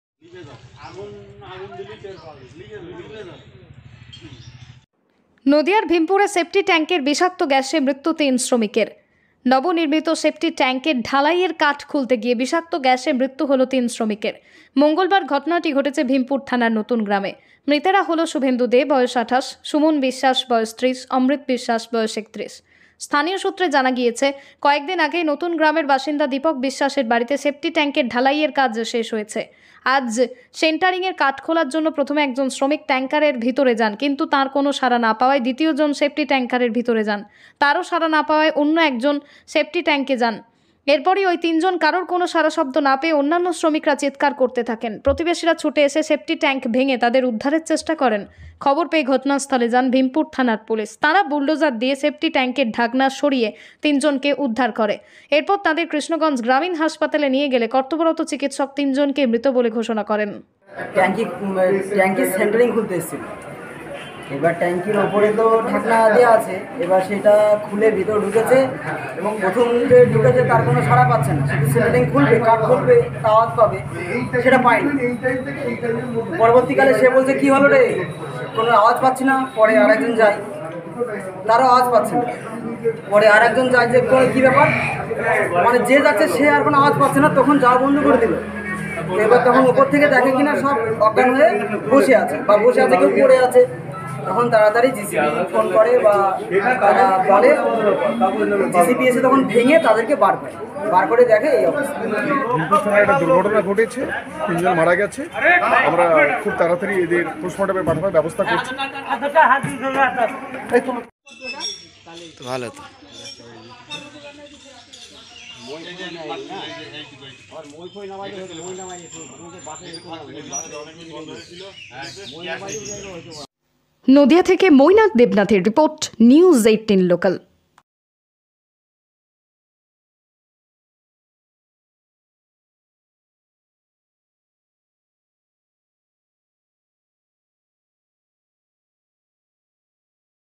Nadia News: সেপটিক ট্যাঙ্কের ভেতরে ঢুকে ঢালাইয়ের কাঠ খুলতে যাওয়াই কাল হল তিন শ্রমিকের
সেন্টারিং এর কাঠ খোলার জন্য প্রথমে একজন শ্রমিক ট্যাংকের ভিতরে যান। কিন্তু তাঁর কোনও সাড়া না পাওয়ায় দ্বিতীয় জন সেপটিক ট্যাংকের এর ভিতর ঢোকেন, তারও সাড়া না পাওয়ায় আরও এক জন নামে ওই সেপটিক ট্যাংকে,